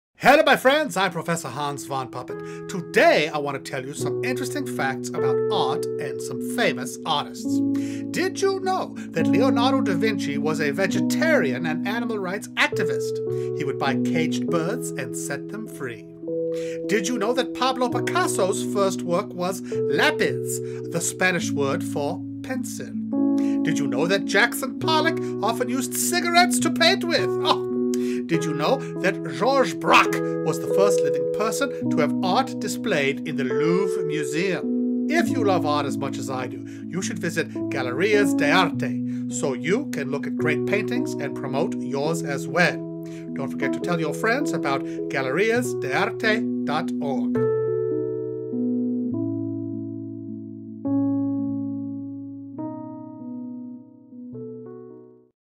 Proffesor Hans Von Puppet Talks About Art
Professor Hans Von Puppet tells us some interesting facts about art, paintings & artists.  He also presents Galerias de Arte